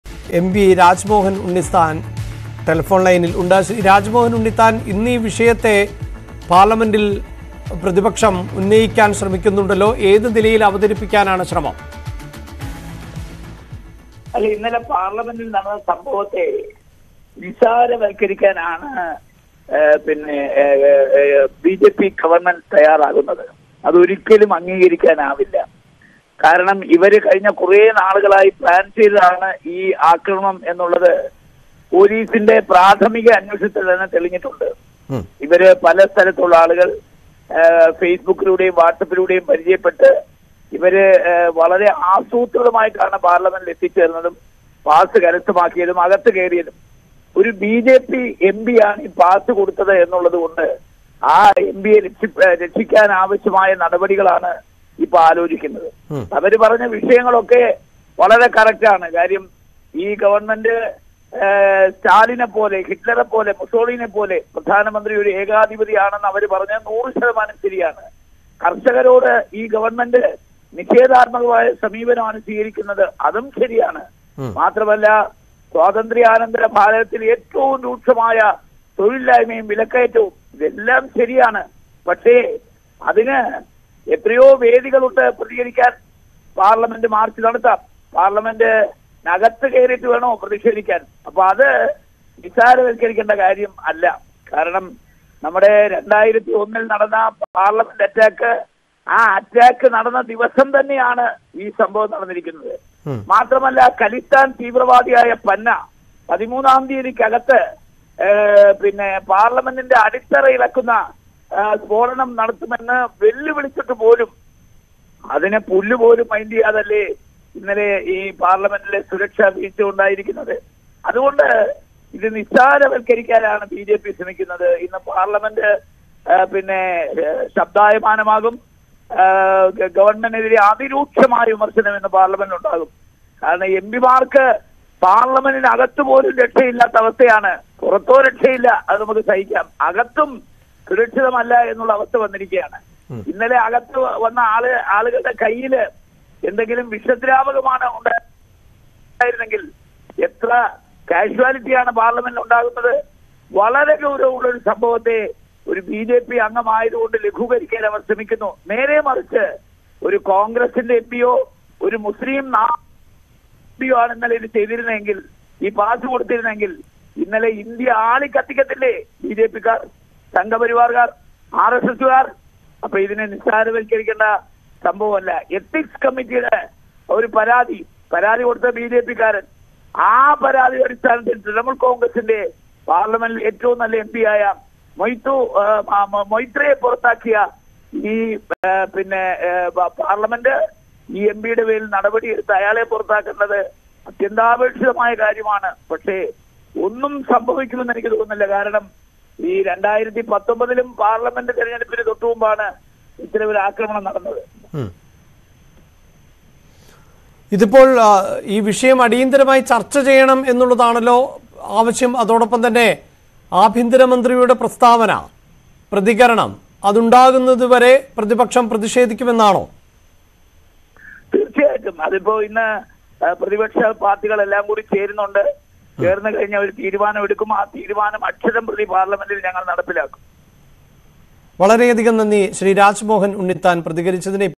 Lok Sabha Security Breach | "പ്രതികൾ കേന്ദ്രസർക്കാരിനെ കുറിച്ച് പറഞ്ഞതെല്ലാം ശരി"; Rajmohan Unnithan
Lok Sabha Security Breach: Parliament അതിക്രമ കേസിലെ പ്രതികളെ ചോദ്യം ചെയ്യും. സർക്കാർ നയങ്ങളോടുള്ള എതിർപ്പാണ് പ്രതിഷേധങ്ങൾക്ക് കാരണമെന്ന് പ്രതികൾ പറഞ്ഞിട്ടുണ്ട്. പ്രതികൾ Facebookലൂടെയാണ് പരിചയപ്പെട്ടതെന്ന വിവരം പുറത്തുവന്നു.